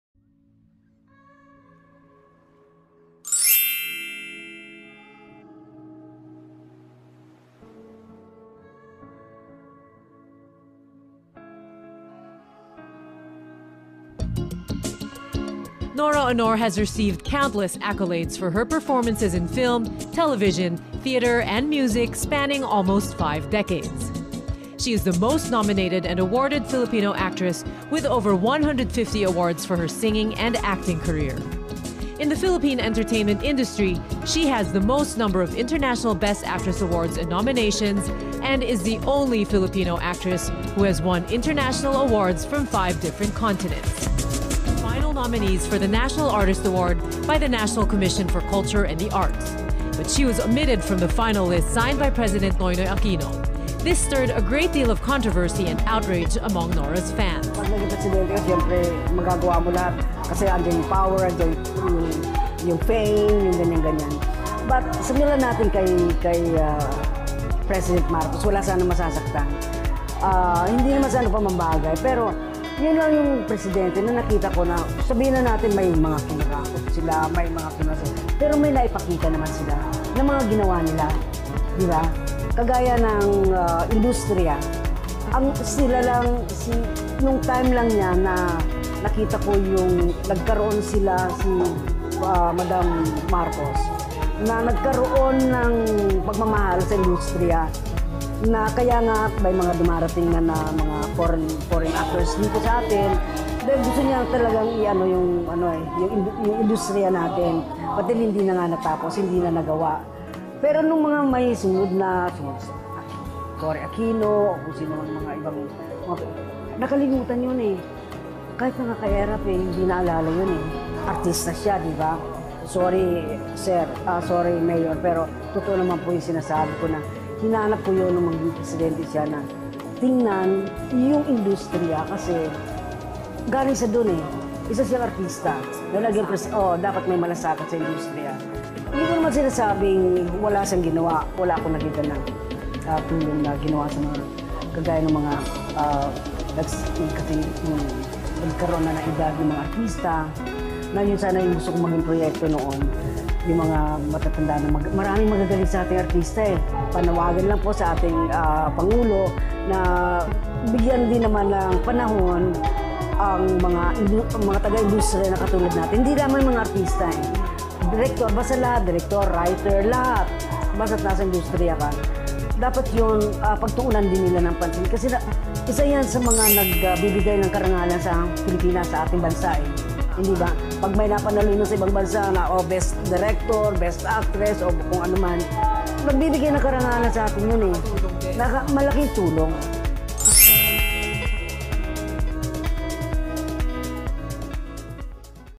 Marcos Lang - Nora Aunor
NORA AUNOR
Sino Ang Presidente nya sa 2022

Marcos lang ang nagmahal sa Industriya-Nora

Kaya wala ng itatanggi na si Nora Aunor na talaga ang pinagpala.

credits to CNN news

using video clips for fairuse

"commenting on this video are turn off at the moment "

Godbless all!